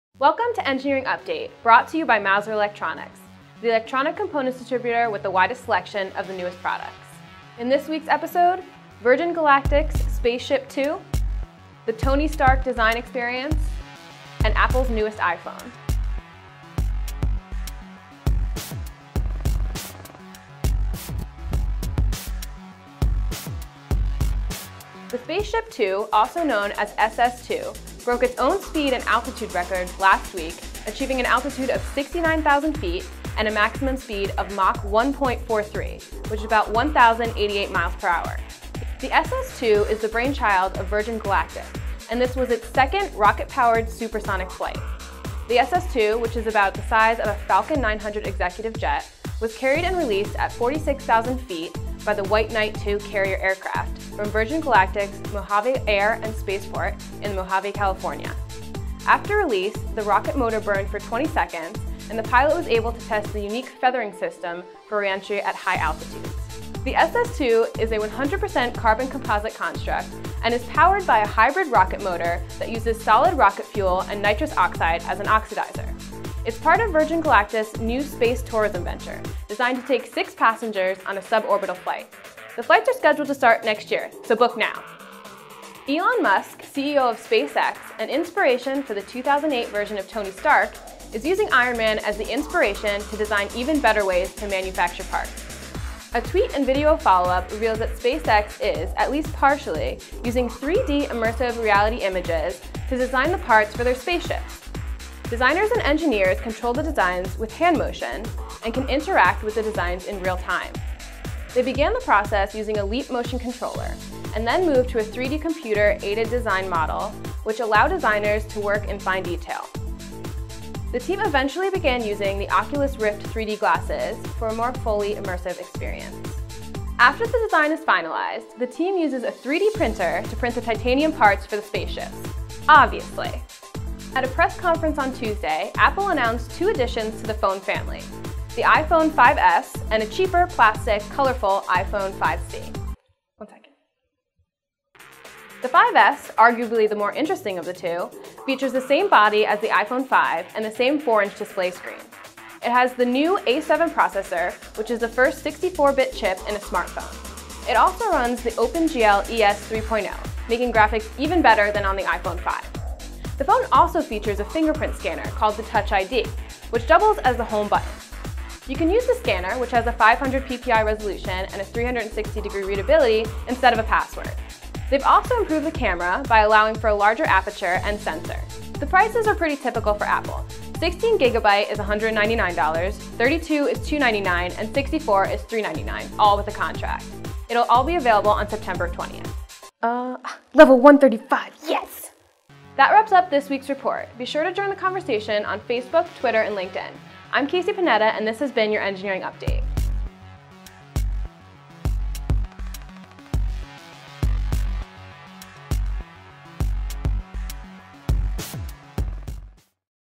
Engineering Update #24: Virgin Galactic's SS2 hits Mach 1.43 and Elon Musk becomes Ironman
SpaceShipTwo also known as SS2 broke its own speed and altitude records last week achieving an altitude of 69,000 feet  and a maximum speed of Mach 1.43, which is 1,088 miles per hour

Elon Musk, CEO of SpaceX and inspiration for the 2008 version of Tony Stark, is using Iron Man as the inspiration to design even better ways of manufacturing parts.

Apple announced two additions to the phone family: the iphone 5s and a cheaper, plastic, colorful iPhone 5c. The 5S, arguably the more interesting of the two, features the same body as the iPhone 5 and the same 4inch display. It has the new A7 processor which is the first 64 bit chip in a smart phone.